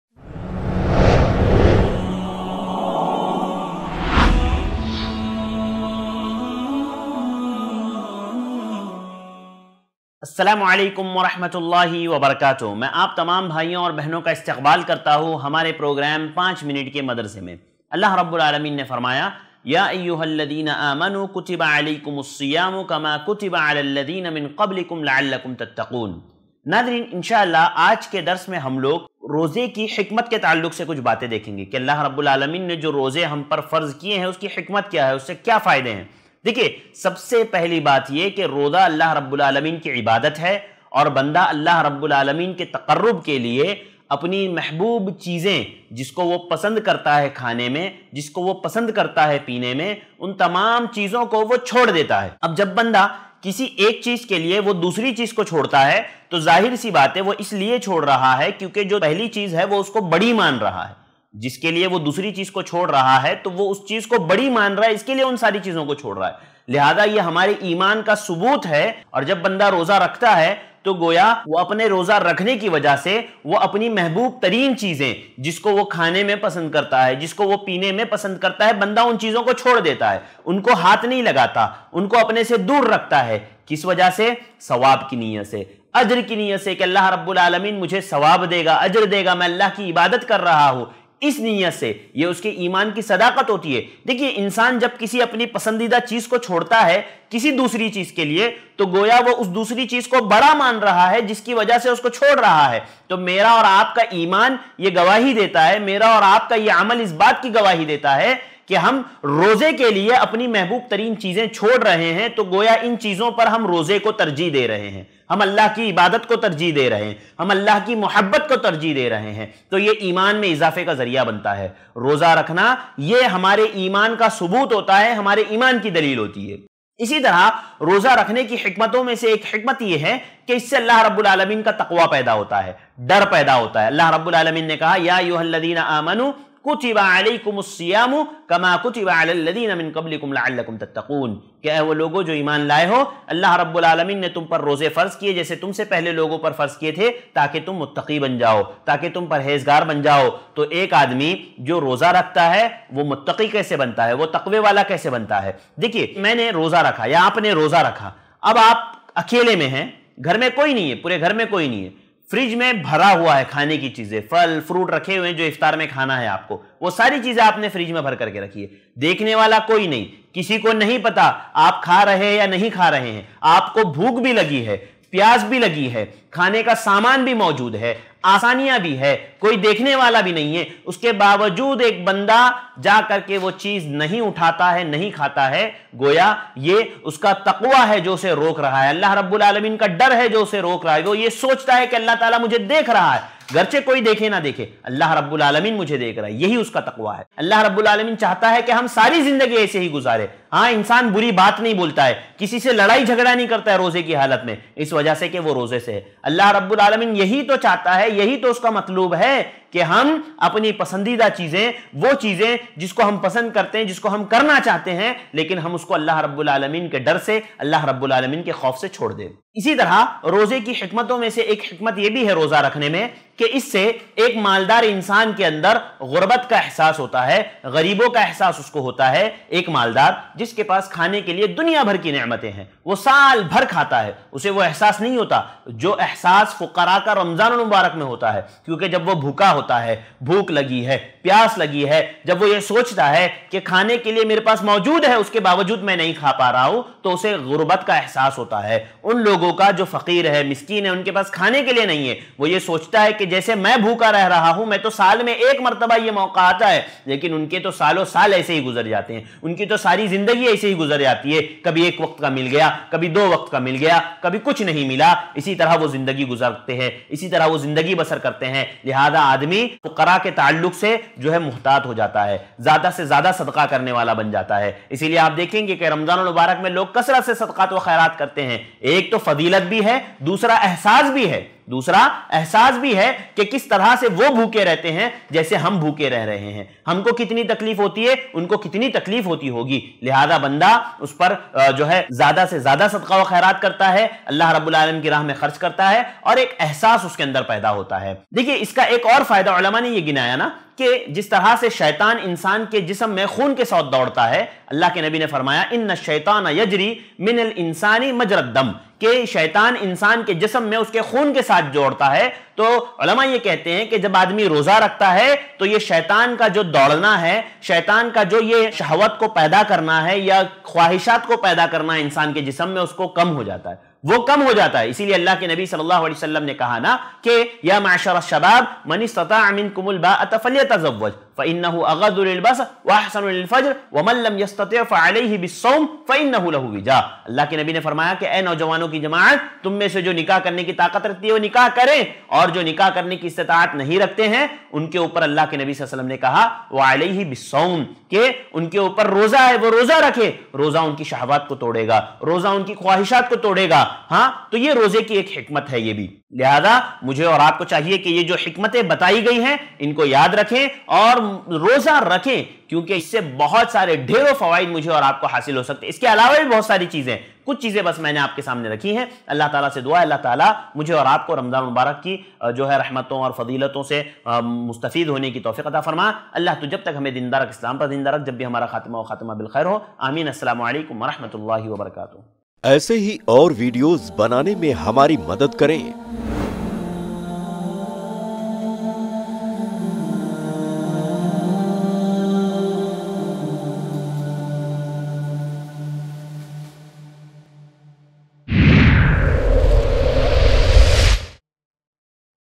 Roze Ke Peeche Ki Hikmatein Aur Uske Fayde | Ramzan Special 2023 - 5 Minute Ka Madarsa 06 | Arshad
Roze Ke Peeche Ki Hikmatein Aur Uske Fayde | Ramzan Special 2023 - 5 Minute Ka Madarsa 06 | Shaikh Arshad Hussain Mohammadi | iPlus TV

UPI : donateilm@icici